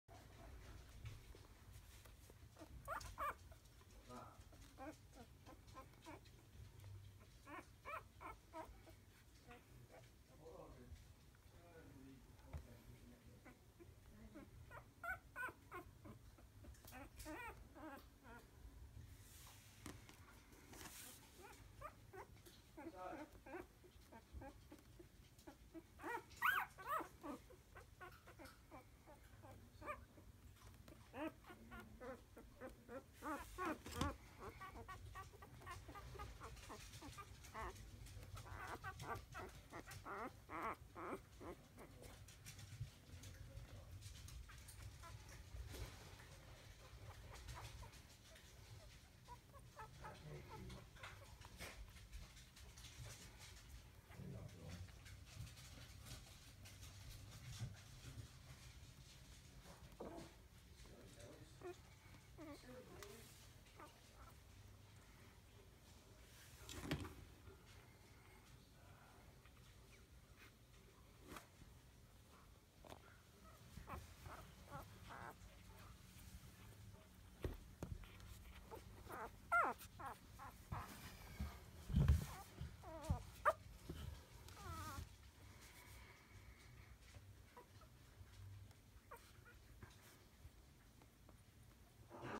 Our Newest Litter of Chocolates
Reese's Babies were born on Jan. 9, 2017. One chocolate and white parti female named Lucy and one chocolate and white parti male named Mocha.